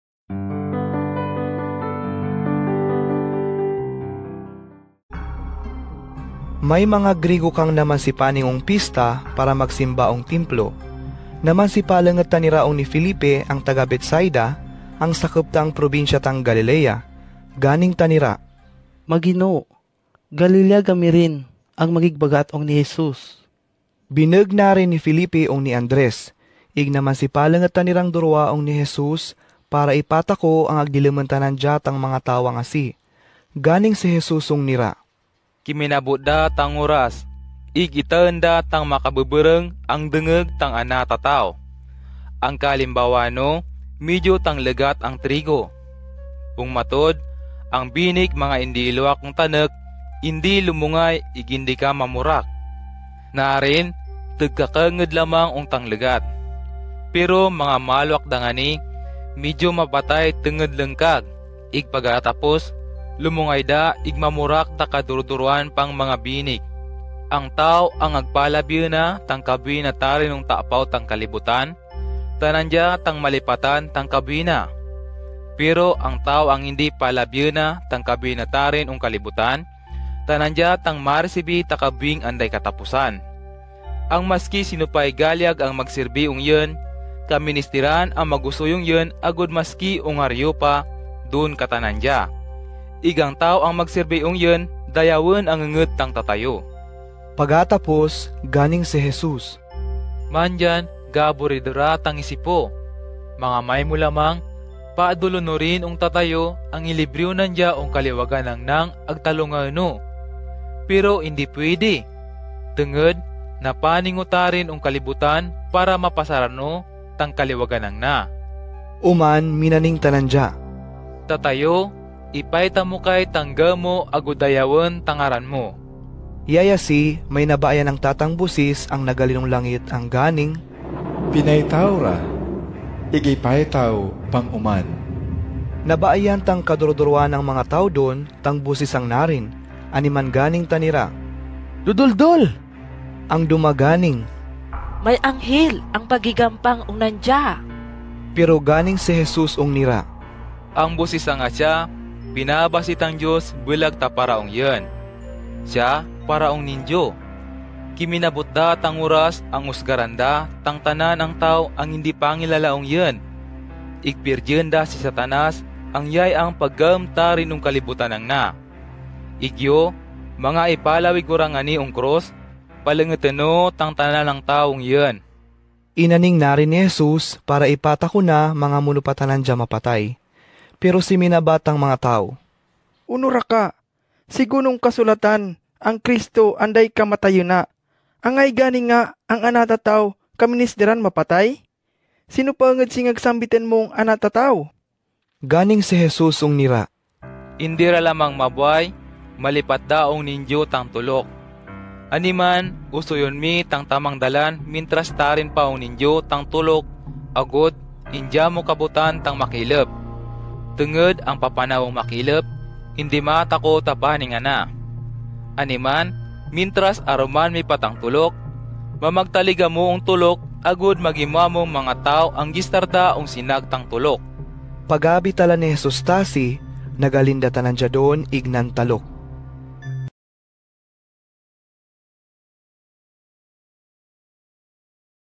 Agutaynen - John 12:20-36 “Jesus foretells his death” [agn]
Scripture Videos from the Gospel of John in Agutaynen of the Philippines. [agn]